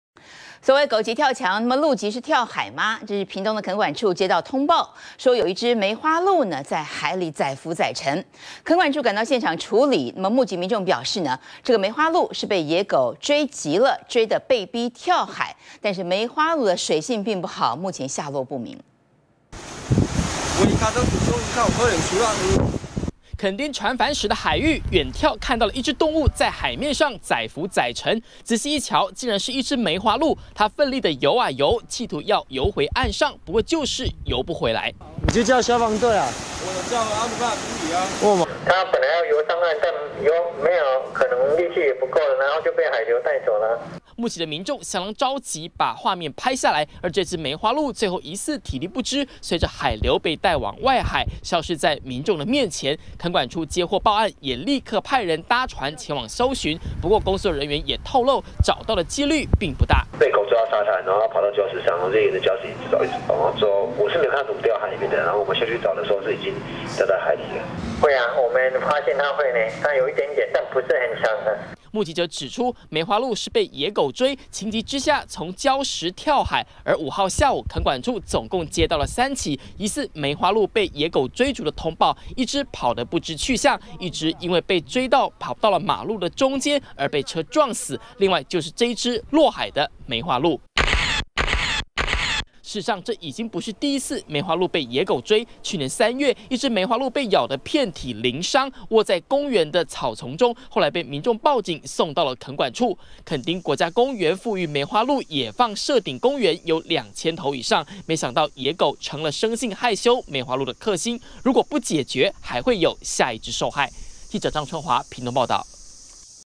20150105中天新聞 野狗追趕跳水逃？ 梅花鹿落海失蹤！
狗急會跳牆，鹿急了是跳海？屏東墾管處接獲通報，在船帆石海域，有一隻梅花鹿在海裡游泳，請墾管處立刻到場處理，沒想到不諳水性的梅花鹿，這一跳游不回來岸上，因此隨著海浪漂出外海後下落不明，根據目擊民眾描述，梅花鹿是被野狗追得跳海了，其實之前也發生過梅花鹿被野狗追逐，一次被車撞死，一次失蹤，結果這一次是跳海失蹤，墾丁國家公園好不容易復育梅花鹿成功，卻慘遭野狗追逐致死，讓復育人員很意外。